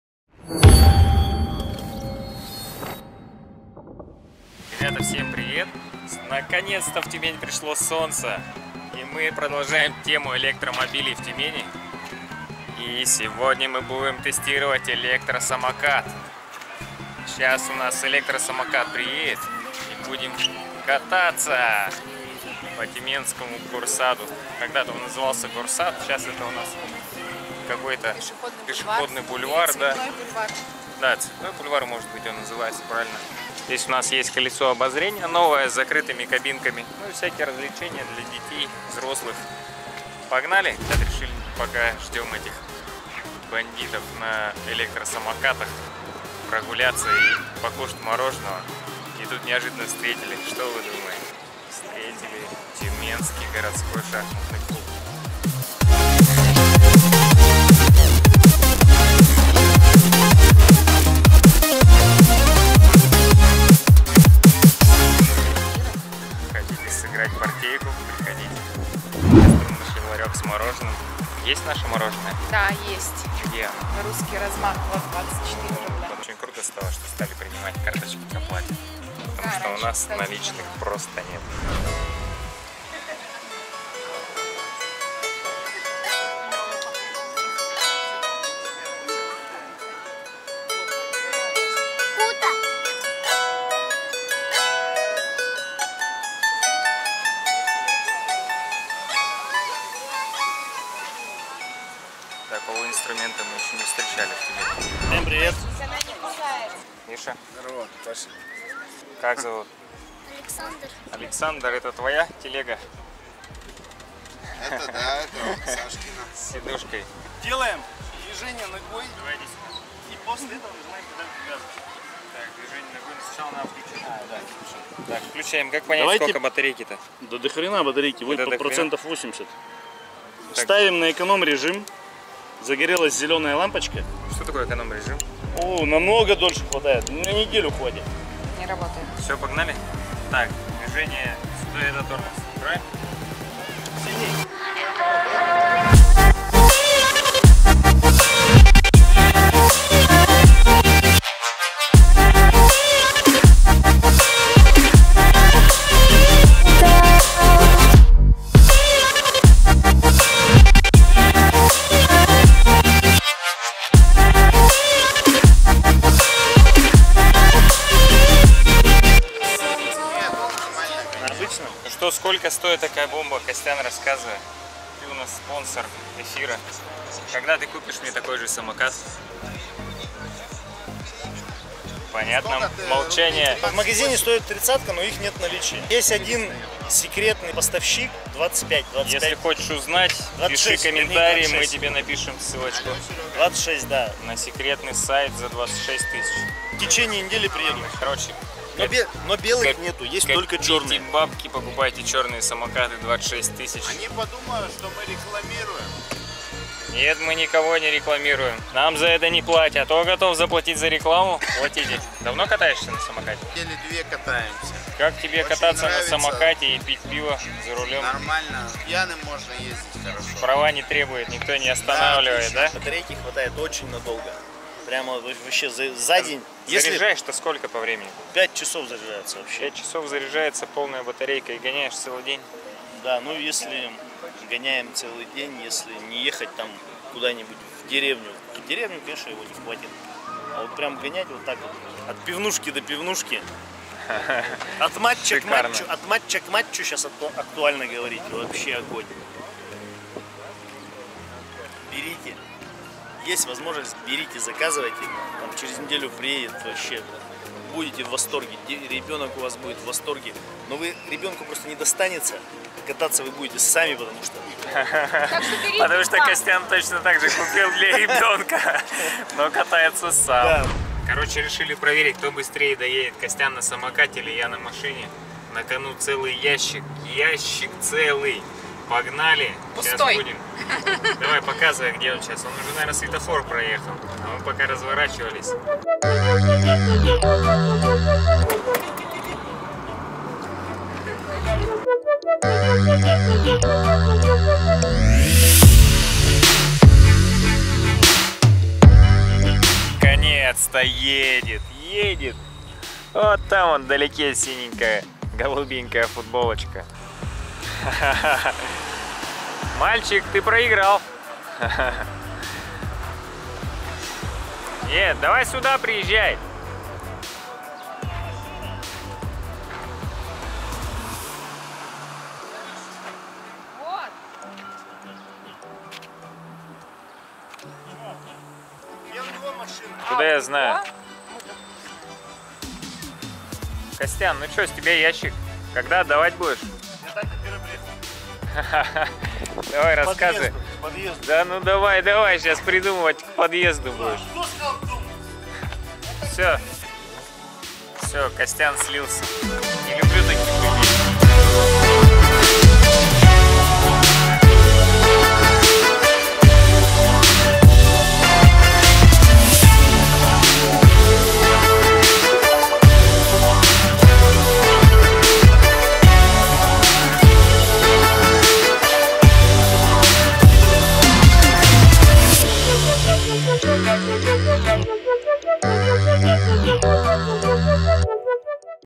Покатушки и Честный обзор Электросамоката XIAOMI в Тюмени
Решили собраться, покататься и сделать честный обзор на самокат. А так же покушать мороженое на цветном бульваре.
Поехали) MikhailPetroff Все билеты я покупаю на - Путешествие МоЁ Искусство.